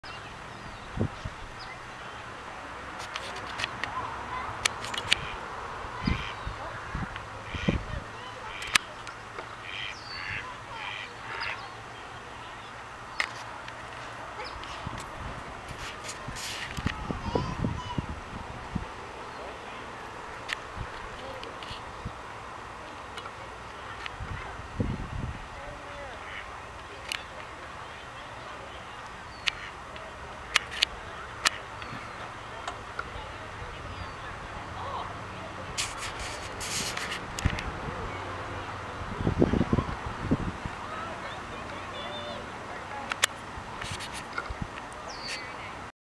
Demonstrating 720P video from Canon PowerShot SD780 IS
Good 720P video ONLY with fixed focus and no zooming